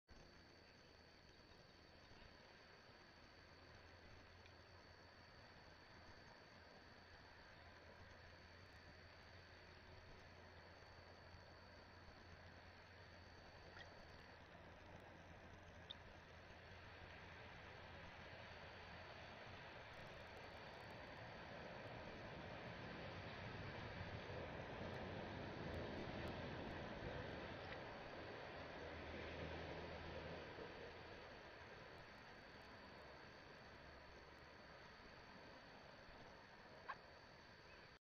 Frogs practising The Lift
These two looked like they were practising the lift from Dirty Dancing 😆